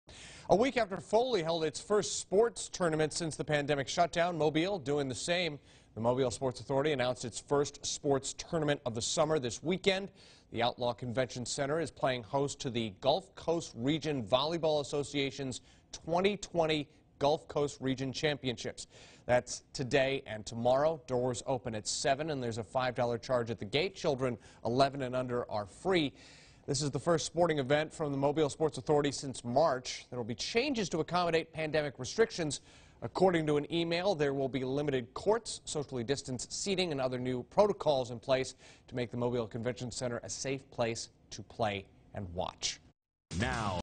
Volleyball tournament in Mobile
It's the first large event from the Mobile Sports Authority in three months